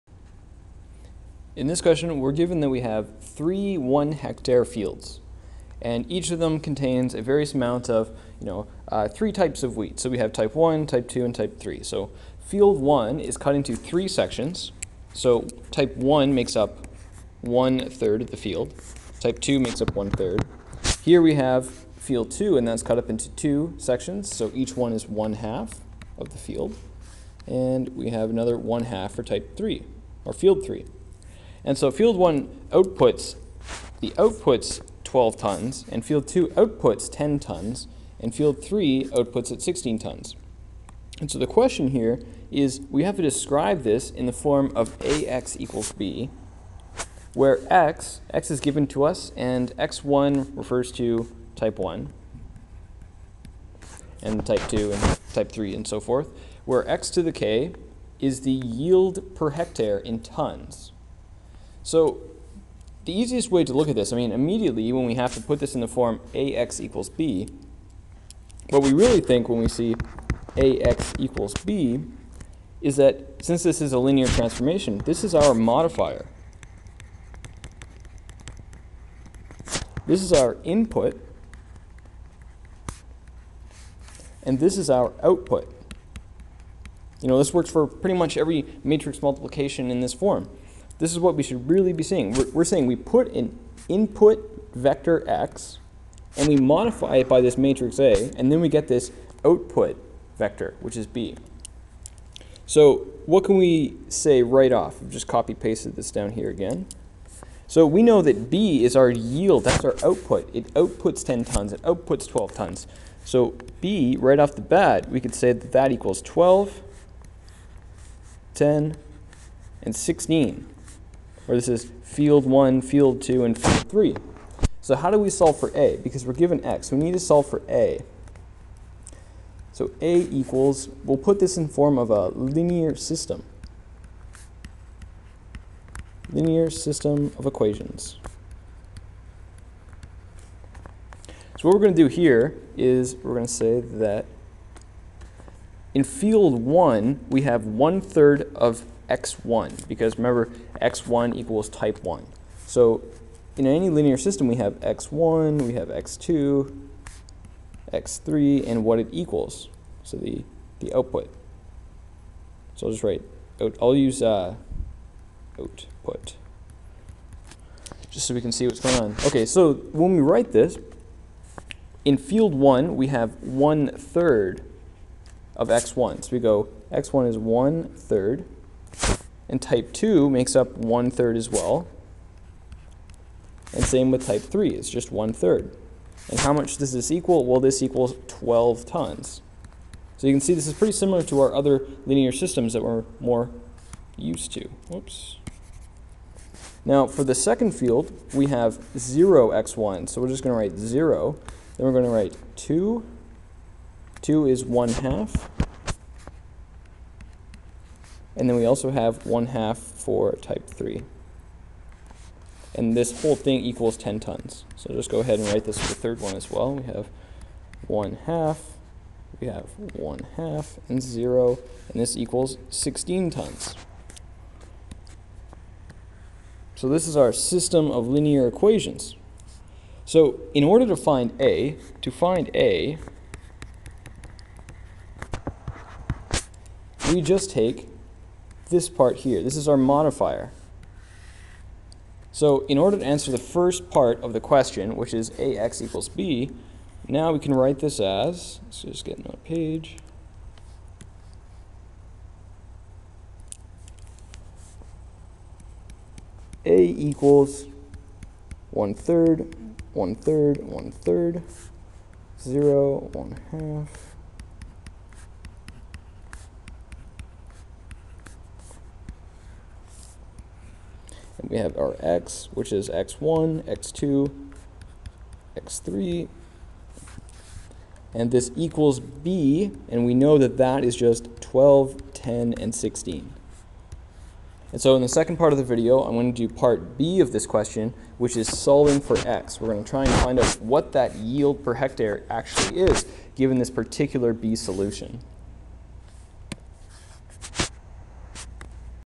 Matrix Multiplication on the form of Ax=B [Linear Algebra] [Part1]
How to approach a problem dealing with matrix multiplication in the form Ax=B. The second part of the question is solved in this video